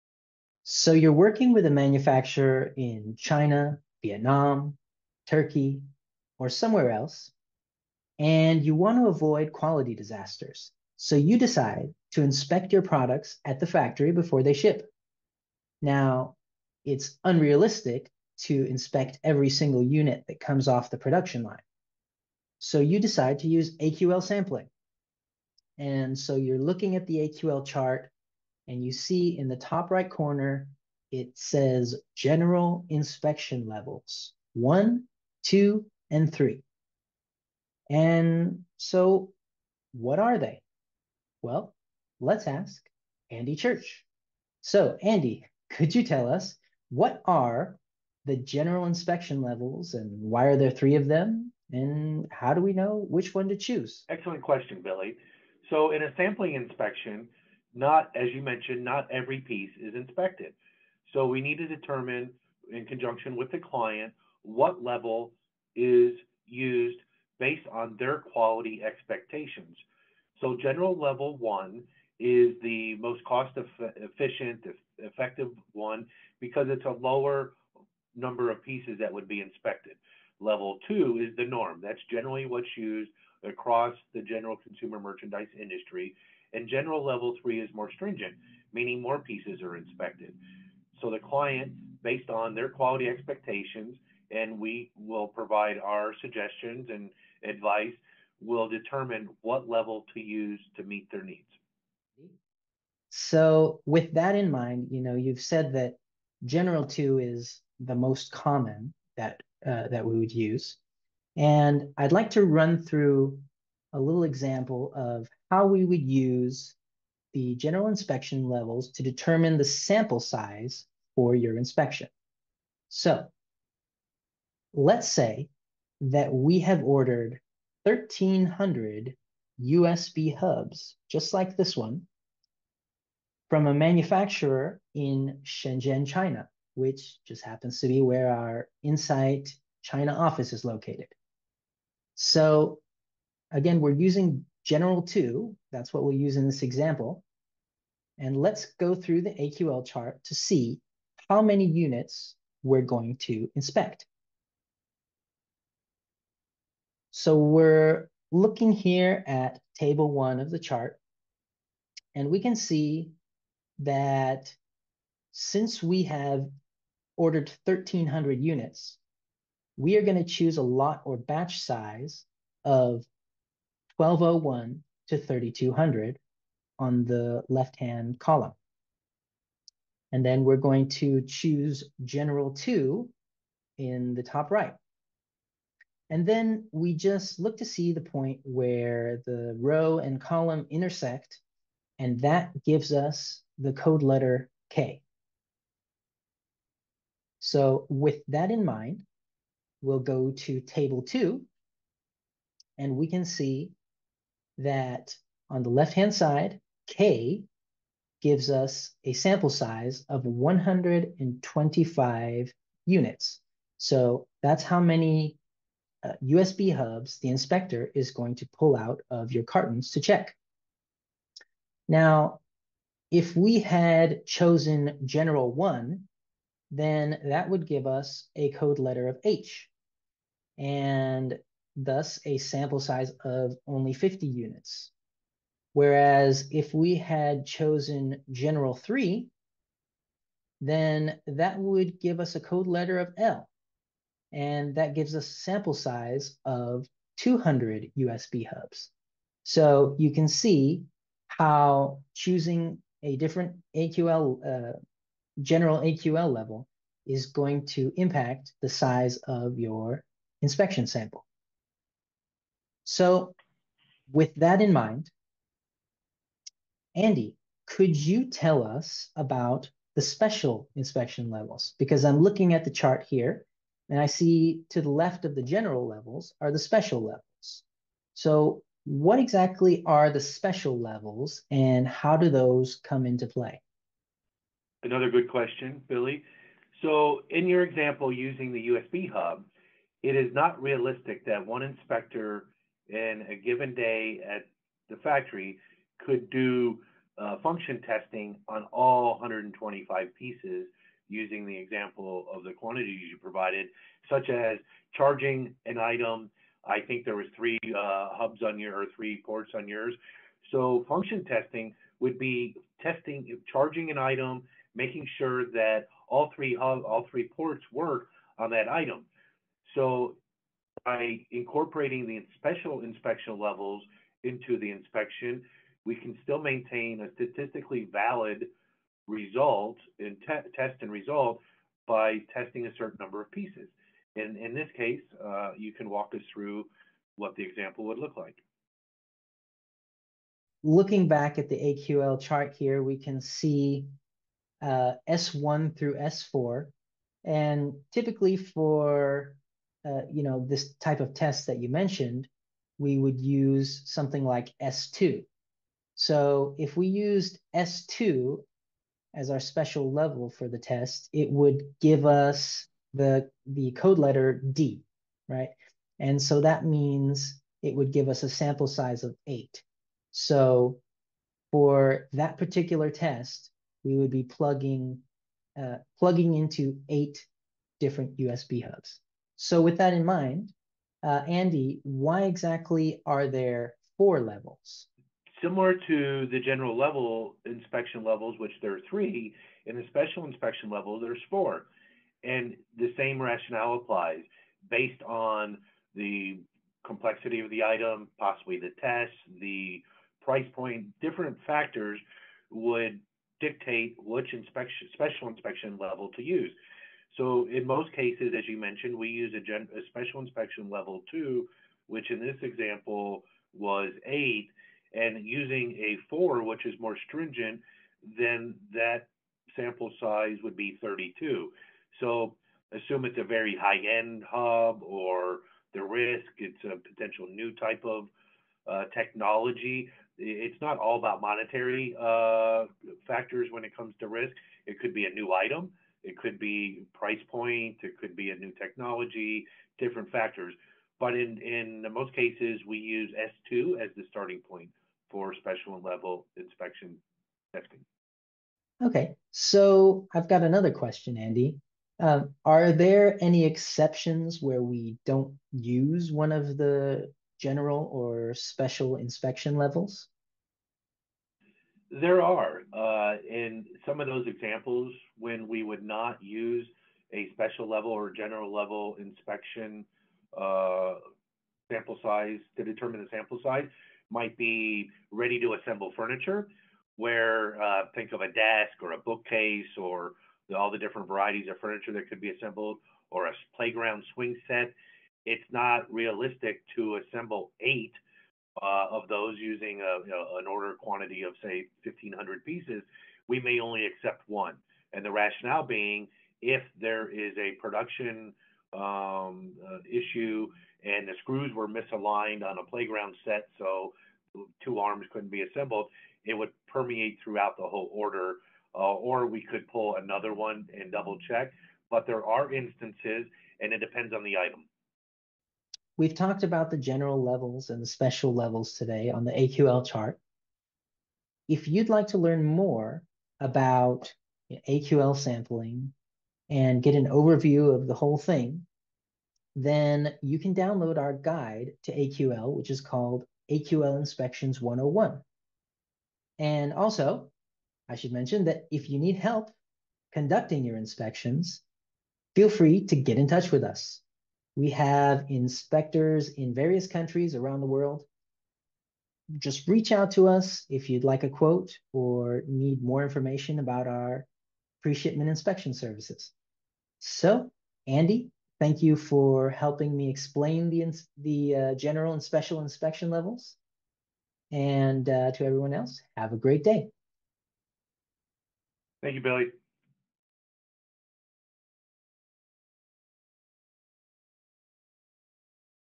How to Use the AQL Inspection Levels
Learn how to use the General and Special Inspection Levels from the AQL chart.